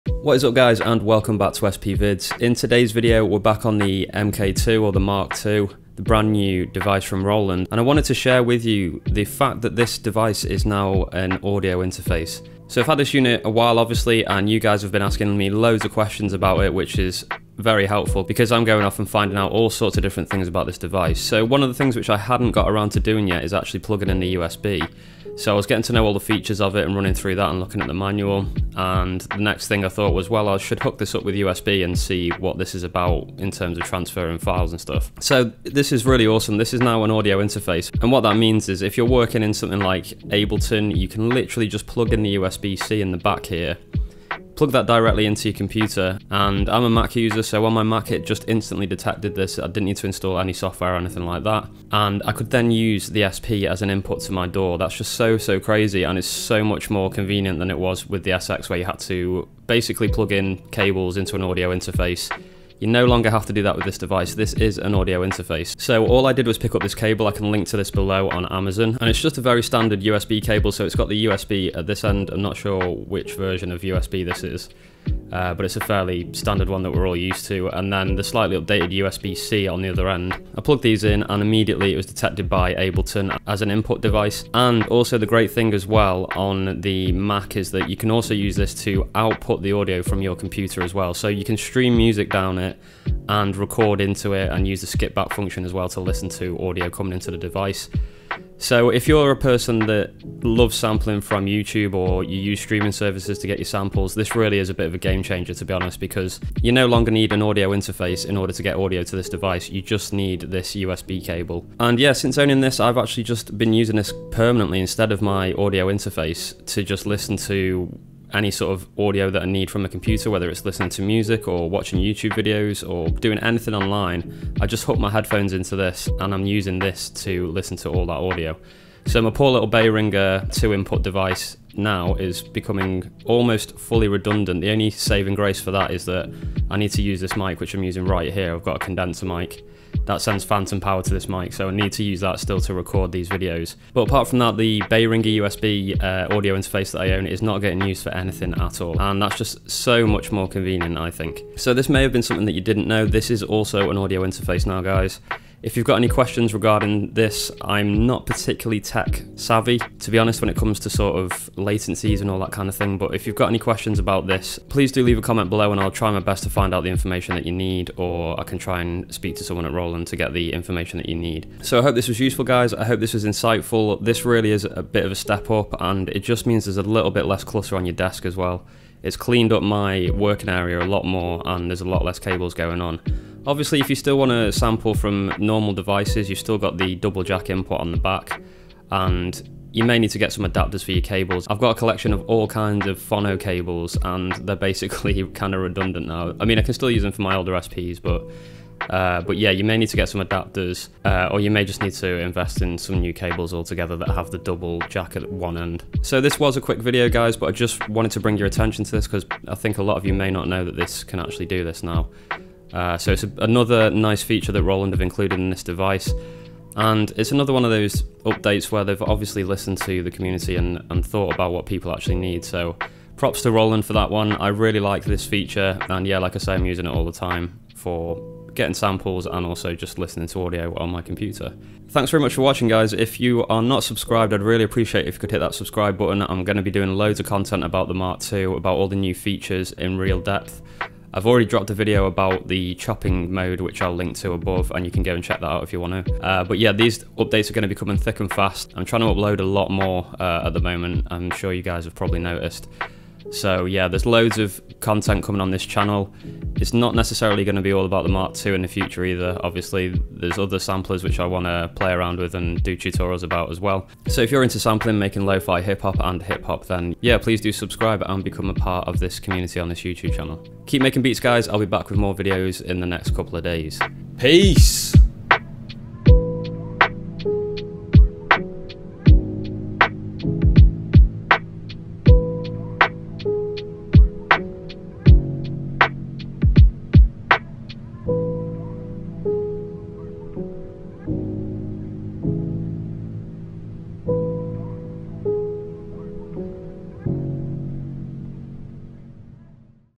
The New SP404 MK2 is also an audio interface!
In this video I wanted to show you guys how you can use the SP404mk2 as an audio interface using just one cable. I think this is  a real game changer and I'm not sure many have spoken about this yet! Let me know what you think about this feature in the comments below